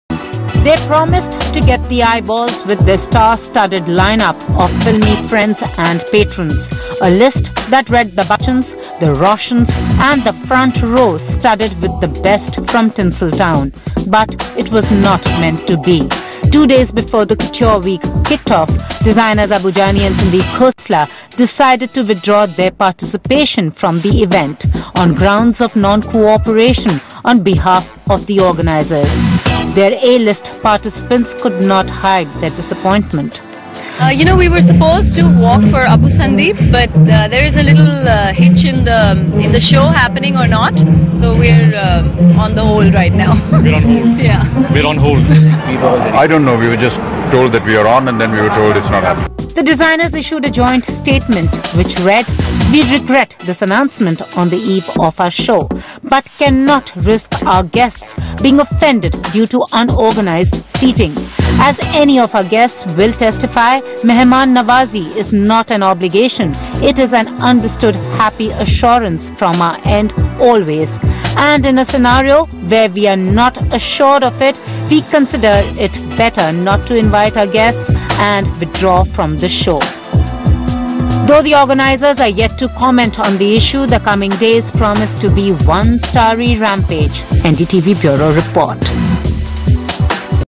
Susanne & Hrithik Roshan - were Supposed to walk for Abu Jani Initially @ Couture Week Oct 2009
Susanne & Hrithik Roshan - were Supposed to walk for Abu Jani Initially @
HDIL India Couture Week with Couture Jewellery Show